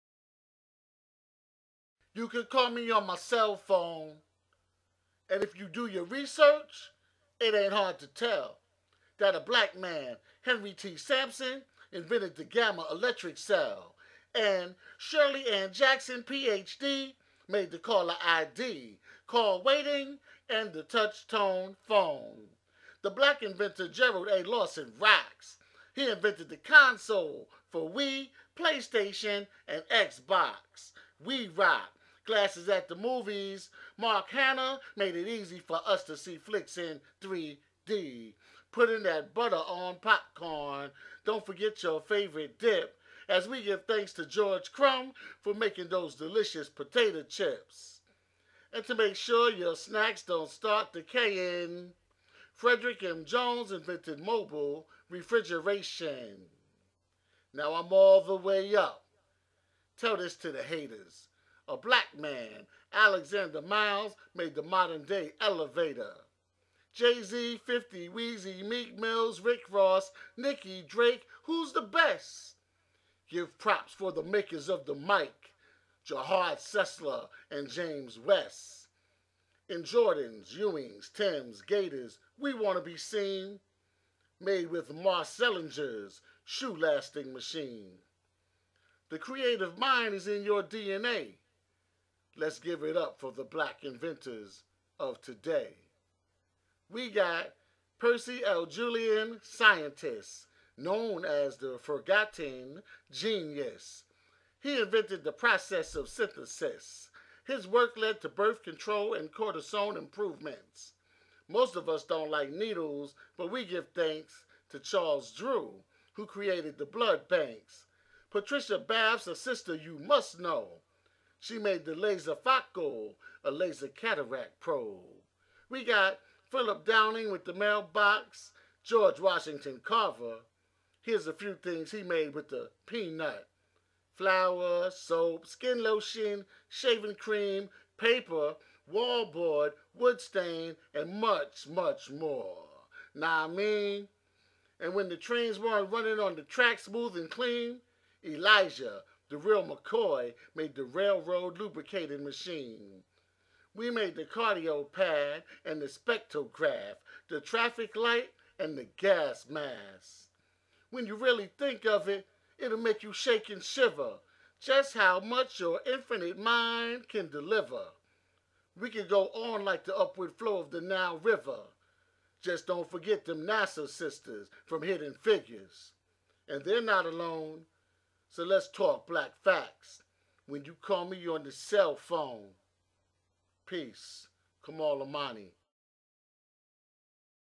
Black Inventions Spoken Word by Kamal Imani-The Hip Hop Teacher
Listen to some of the inventions that most people don't know are made by Black/African/Indigenous  people in America. Spoken Word Poetry by Kamal Imani aka Kamal The Hip Hop Teacher!